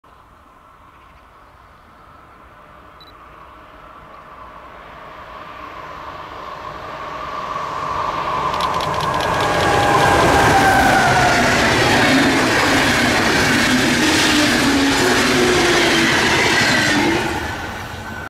[50 Jahre IC] DB Fernverkehr 101 013-1 mit dem IC2005 in Gau Algesheim
Am 16.10.21 konnte ich in Gau Algesheim die 50 Jahre IC 101 im IC Design mit dem IC2005 Filmen auf dem Weg nach Karlsruhe